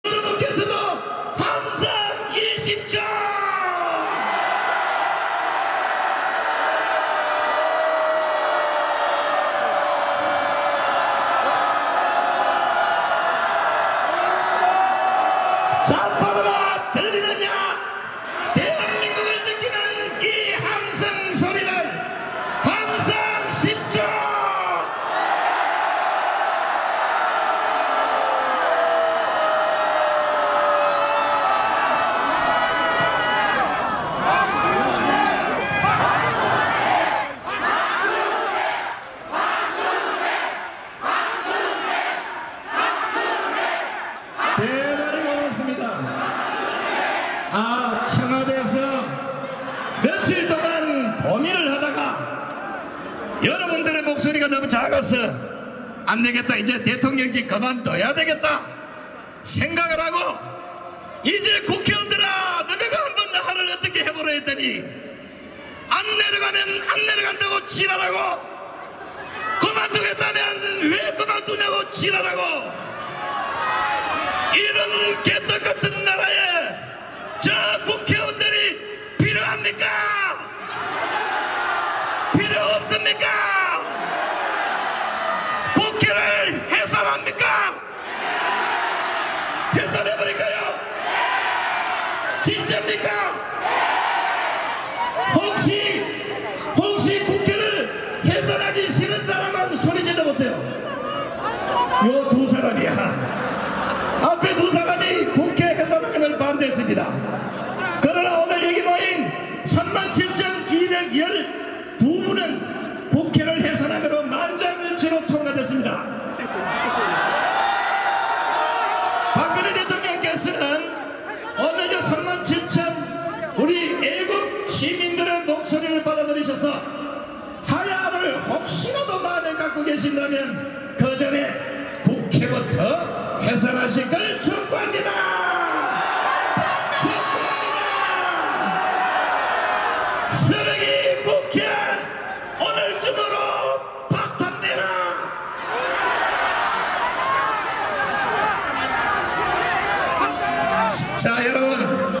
[헌정수호 동대문 대회] 손상대 : 국회 해산돼야 한다
2016년 12월3일 오후 2시부터. 박사모 등 보수대연합 주최 동대문 헌정수호 국민대회 연설.